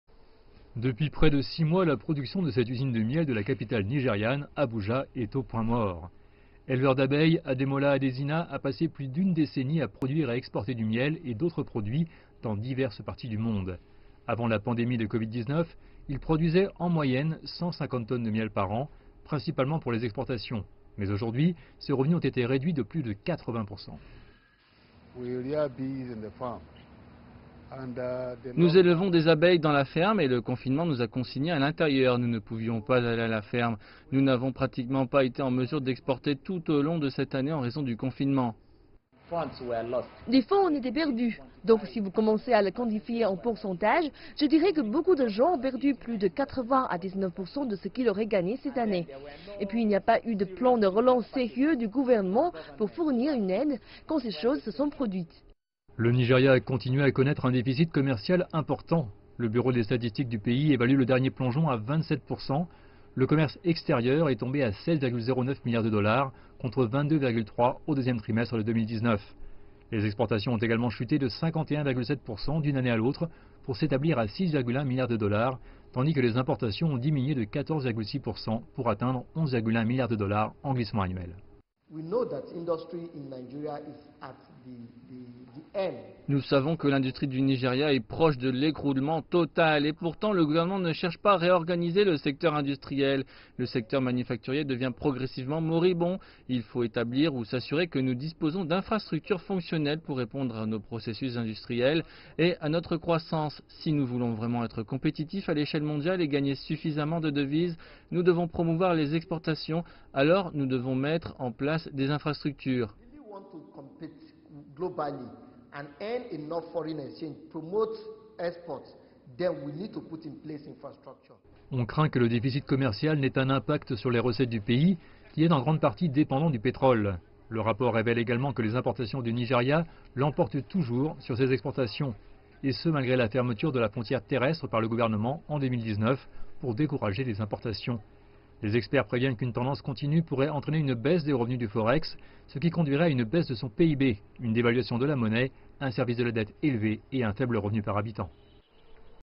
Nigéria : Le commerce extérieur plonge de 27% au 2ème trimestre
Le commerce extérieur du Nigeria a continué à connaître une spirale descendante au cours du trimestre clos en juin. Il s’agit de la troisième baisse consécutive pour le pays depuis 2019. Le dernier rapport du Bureau des statistiques montre que le commerce extérieur du Nigeria a plongé de 27% au
deuxième trimestre de l’année, avec un déficit commercial s’élevant à 16,1 milliards de dollars. La baisse a été alimentée par les chaînes d’approvisionnement perturbées par le COVID-19 ainsi que les fermetures de frontières.